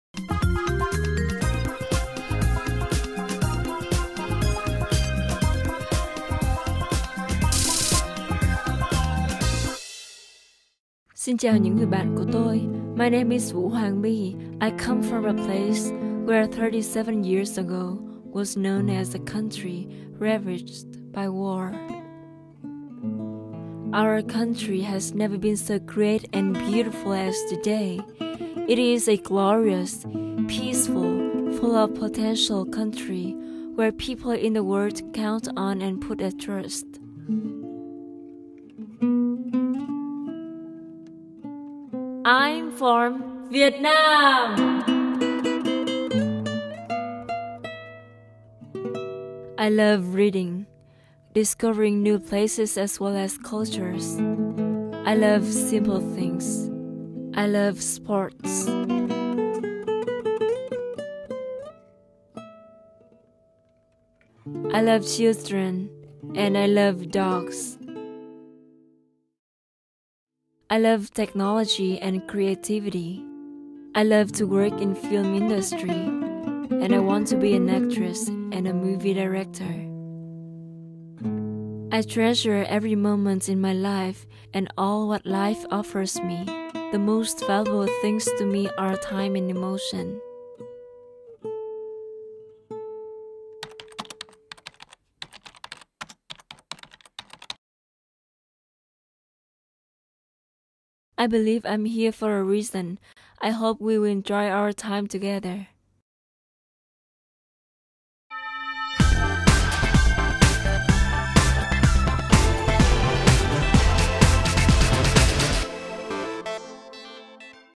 2012 Miss World Profile - Vietnam - NhaSing.com
Vu Hoang My - Miss World 2012 Contestant from VIETNAM

Hoangmy is a multimedia design student and aspires to become a movie director. She likes reading, writing, watching movies and travelling. She describes herself as a passionate, goal-orientated, creative and spontaneous person.